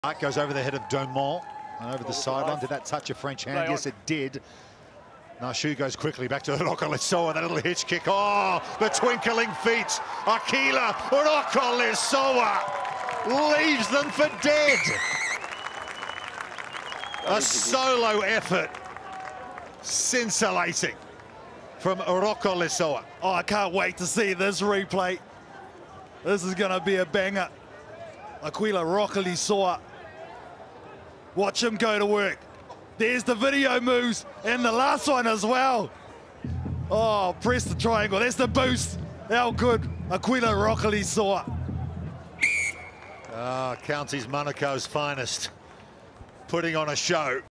The Rocketman breaking ankles to score INCREDIBLE try
NZ Sevens star Akuila Rokolisoa steps his way to the tryline for a wonder try on the sevens circuit. Ankles were broken in the process